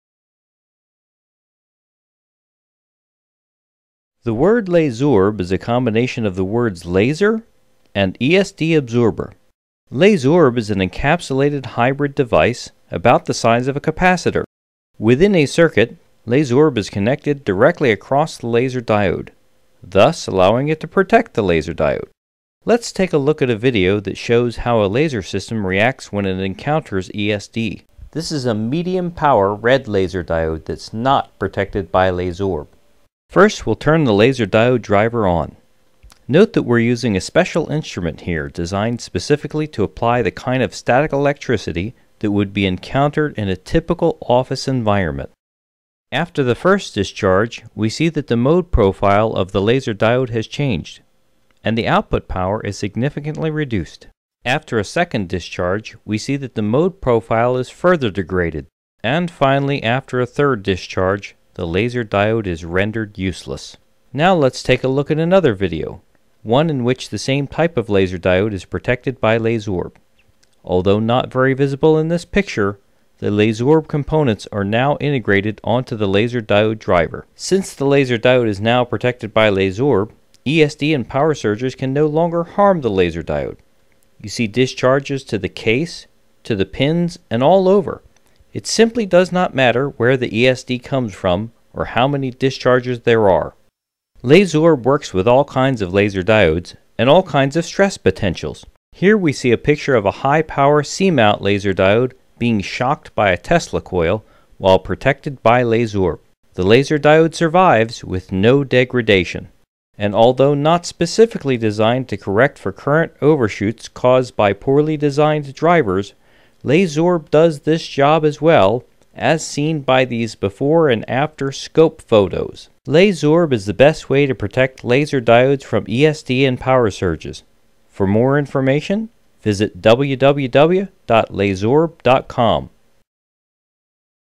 LASORB: ESD and power surge absorber for laser diodes
Laser diodes can easily fail due to electrostatic discharge. LASORB is a small, inexpensive component which provides 100% protection for laser diodes. Unlike current methods, LASORB can protect diodes even from very high voltages such as Tesla coils.